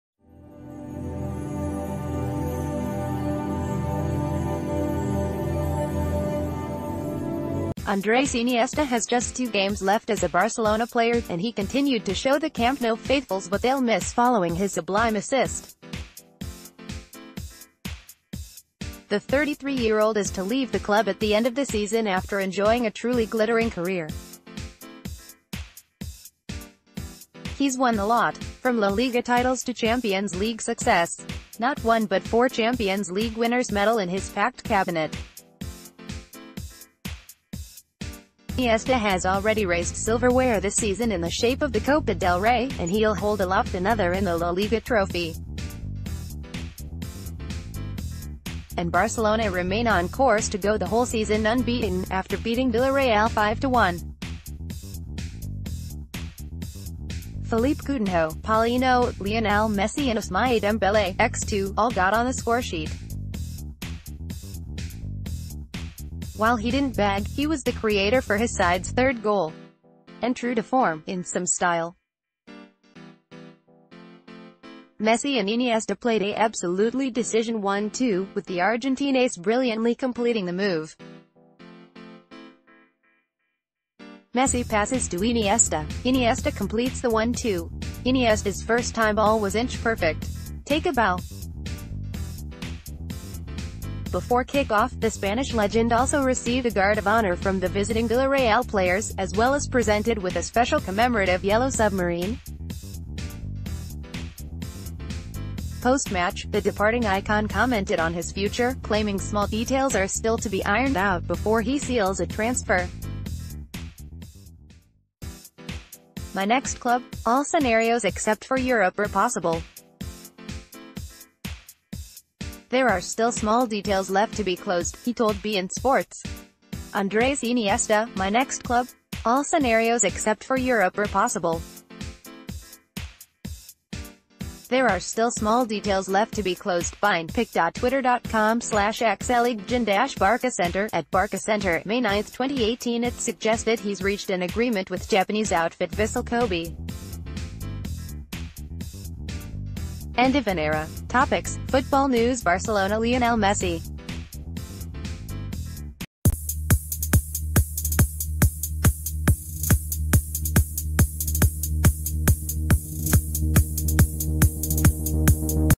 Breaking News | Andres Iniesta Plays Utterly Insane One-Two With Lionel Messi For Brilliant Goal
Andres Iniesta has just two games left as a Barcelona player, and he continued to show the Camp Nou faithfuls what they'll miss following his sublime assist. The 33-year-old is to leave the club at the end of the season after enjoying a truly glittering career. He's won the lot. From La Liga titles to Champions League success. Not one but FOUR Champions League winners medal in his packed-cabinet. Iniesta has already raised silverware this season in the shape of the Copa del Rey, and he'll hold a...